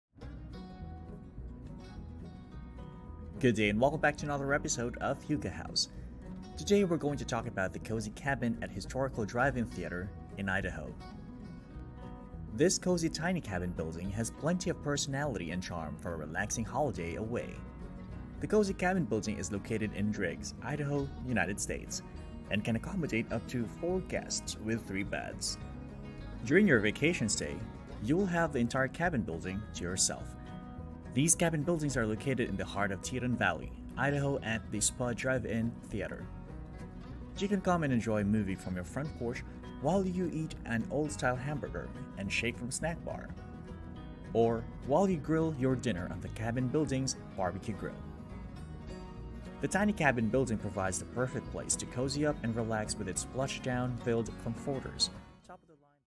Cozy Cabin at Historical Drive in Theatre in Idaho
This cozy tiny cabin building has plenty of personality and charm for a relaxing holiday away. The cozy cabin building is located in Driggs, Idaho, United States and can accommodate up to 4 guests with 3 beds. During your vacation stay, you will have the entire cabin building to yourself. These cabin buildings are located in the heart of Teton Valley, Idaho, at The Spud Drive-in Theater. You can come and enjoy a movie from your front porch while you eat an old-style hamburger and shake from the snack bar, or while you grill your dinner on the cabin building's barbecue grill.

The tiny cabin building provides the perfect place to cozy up and relax with its plush down-filled comforters, top of the line bedding, and ambiance to fill your senses. During your stay you can rest up for a day spent enjoying beautiful Yellowstone National Park and/or in the Grand Tetons. The cabin building units do not include bathrooms, the bathrooms are located inside The Spud Snack Bar Diner, which is about a 20-yard walk from each tiny cabin. The bathrooms are available 24/7. Anyone who comes to the theater also has use of this bathroom. It is a unique situation that guests should be aware of. The showers are also a twenty-yard walk from the cabins, located on the outside, North end of the Diner.

During your stay, you'll have use of the hot tub which is available for all guests to use during their vacation stay. You can park in any spot that is convenient for you near to your tiny cabin or camper. The tiny cabin is on the same property as the Drive-In Theatre, which means there will be movie patrons coming and going from Monday to Saturday. There is a diner which opens Monday to Saturday at 6 pm that you can enjoy and purchase a meal or other treats from during your vacation stay. Movies start about 20 minutes after sunset.

Driggs, Idaho is a quiet town that is far less busy than Jackson, which is just the right pace for the people who live there. The hiking in the summer is excellent, and skiing in the winter is amazing. The bike paths in this location are very user-friendly and offer a great way to get around town on a bike or foot for a walk. There are also some great family-friendly parks around town, and also down the road in Victor too. The local shops in Driggs are cute and fun to browse and shop. Driggs is only 2 miles from the tiny houses and Victor is 6 miles. Both places offer quality and diverse food options. Teton Valley has a well established outdoor scene, with hiking, fishing, and skiing to enjoy. They are located just 45 minutes from Jackson Hole Wyoming, 55 minutes from Grand Teton National Park, 48 minutes from Teton Village and Jackson Hole Mountain Resort, 1 hour and 45 minutes from West Yellow Stone, and 2 hours from the South Entrance of Yellowstone.